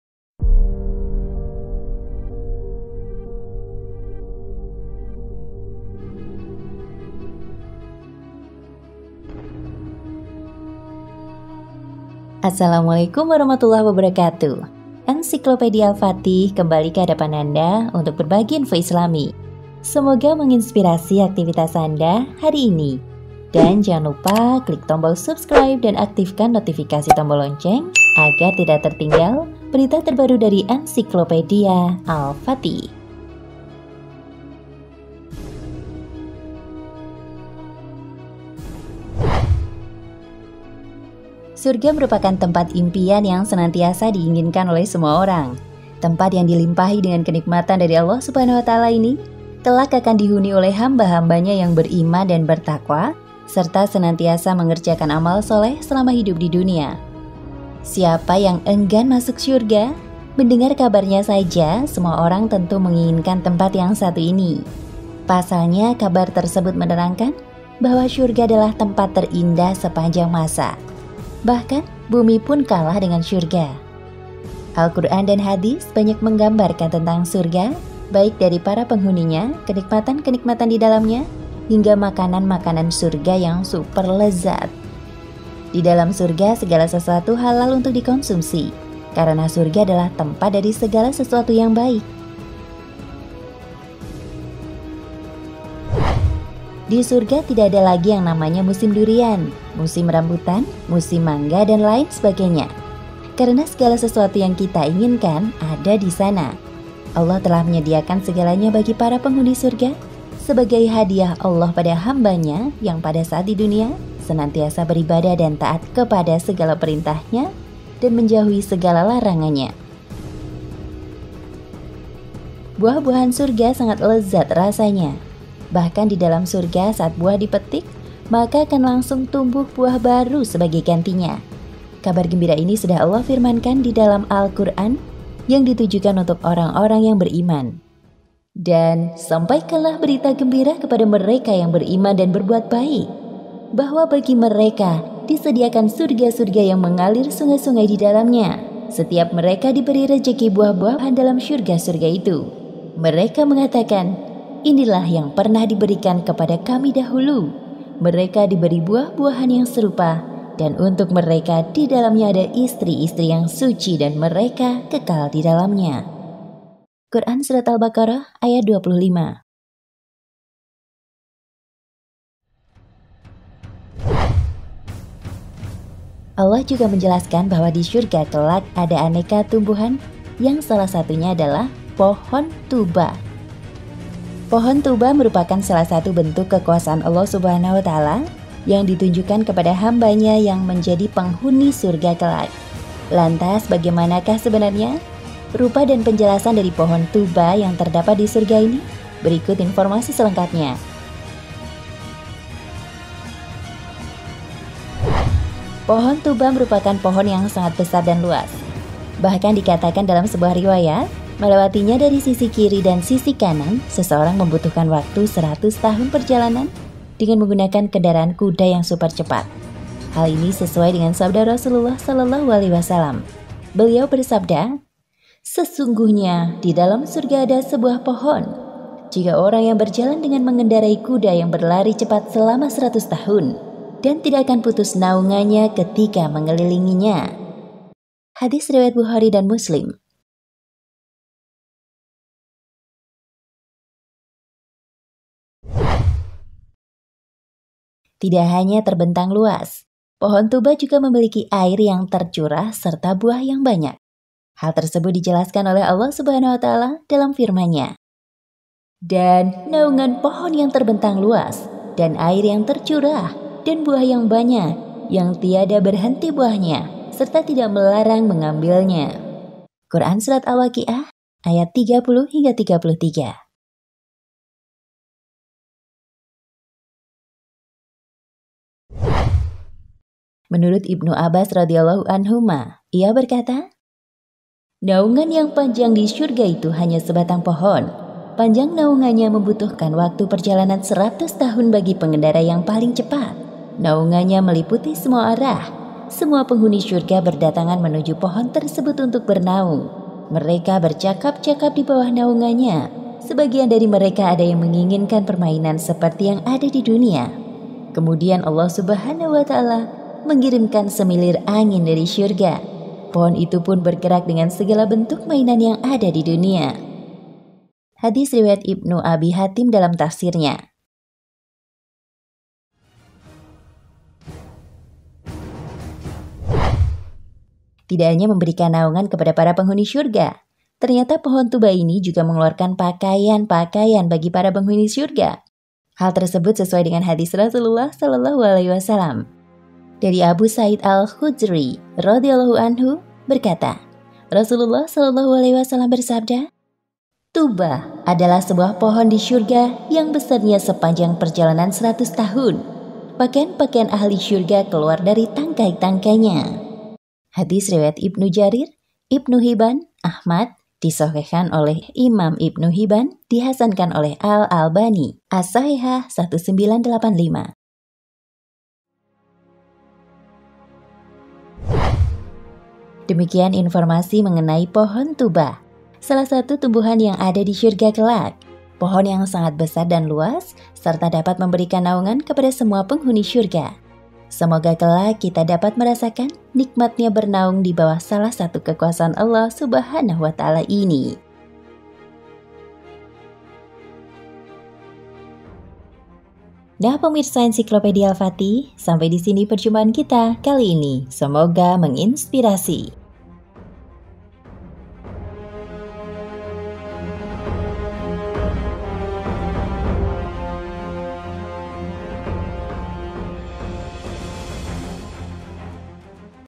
FAKTA POHON TUBA ! SALAH SATU POHON YANG ADA DI SURGA
Pohon tuba adalah salah satu pohon yang ada di surga. Pohon ini memiliki keunikan tersendiri. Mau tau info selengkapnya..? Yuk simak video berikut